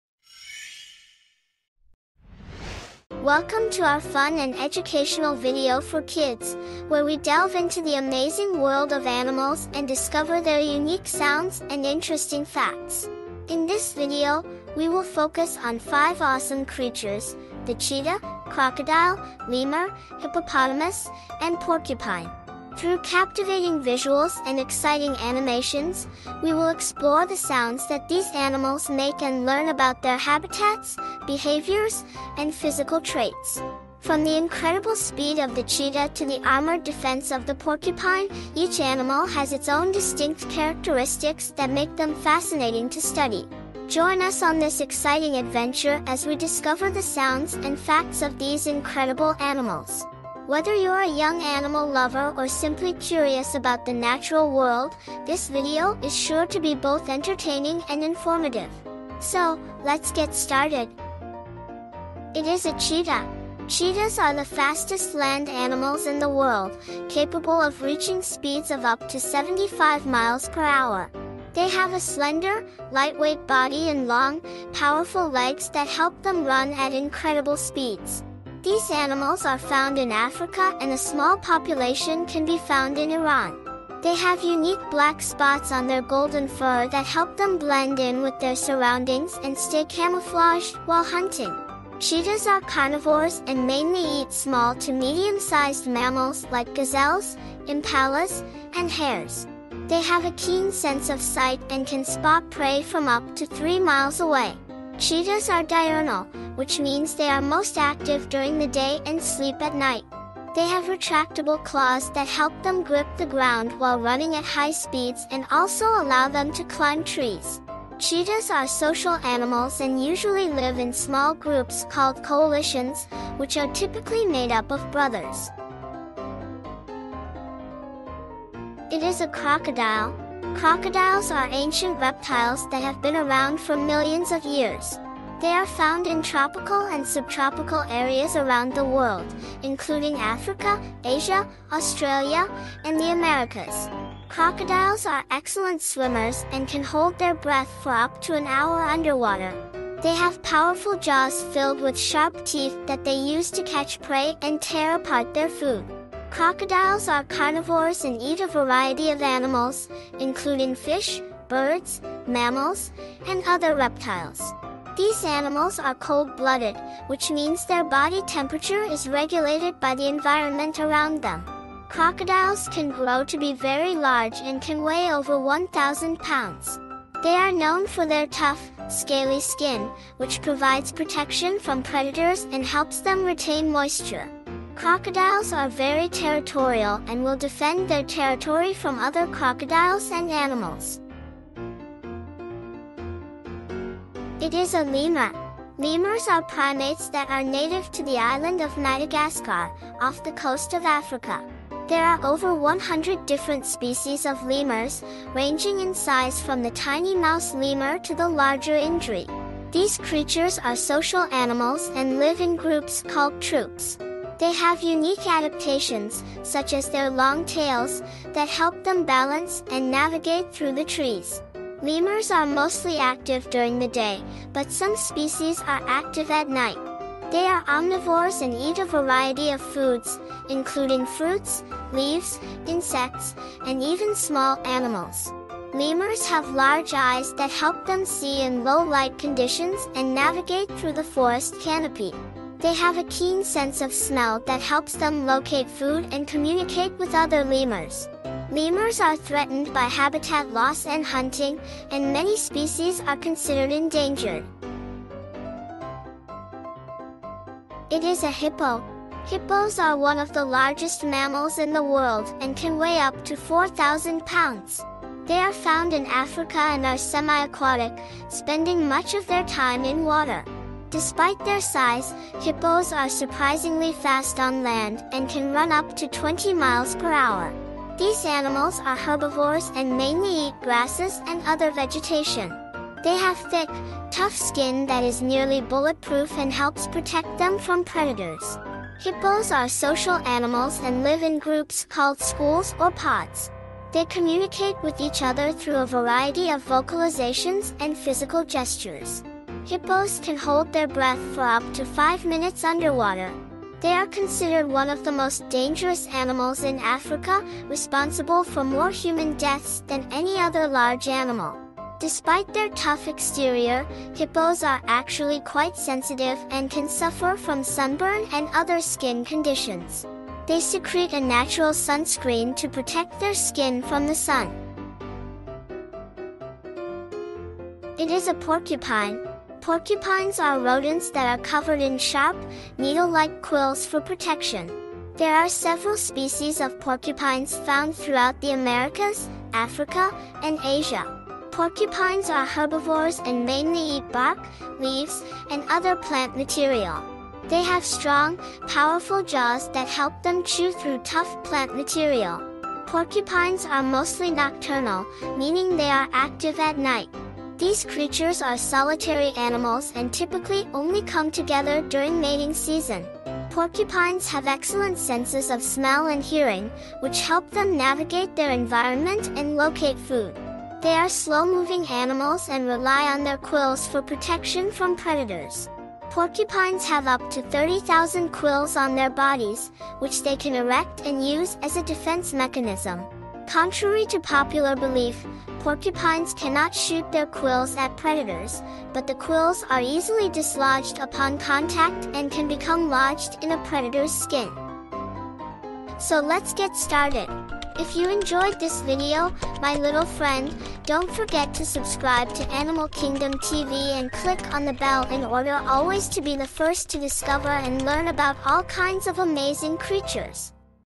Learning wild animals facts video for kids! \ Cheetah, Crocodile, Lemur, Hippo and Porcupine!
Welcome to our exciting video about exploring wild animals! In this video, we will be learning all about some of the most fascinating creatures in the animal kingdom: Cheetah, Crocodile, Lemur, Hippo, and Porcupine.

First up, let's learn about the fastest land animal in the world, the Cheetah! Did you know that a cheetah can run up to 70 miles per hour? That's faster than most cars on the highway! We'll explore the secret behind the cheetah's speed and learn other interesting cheetah facts.

Next, we'll dive into the world of the Crocodile. These fierce predators are known for their strong jaws and sharp teeth. We'll learn how they hunt their prey and how they protect their babies.

Then, we'll swing over to Madagascar to meet the Lemur. These cute primates are the only mammals that can fly! We'll learn about their unique features and how they live in the wild.

After that, we'll splash into the water to meet the Hippo. These massive animals spend most of their time in the water and can hold their breath for up to 5 minutes! We'll learn how they communicate with each other and why they love to roll around in the mud.

Last but not least, we'll encounter the prickly Porcupine. Did you know that porcupines are covered in up to 30,000 sharp quills? We'll learn how they use these quills to protect themselves from predators and how they live in the wild.

We hope you enjoyed learning all about these amazing animals!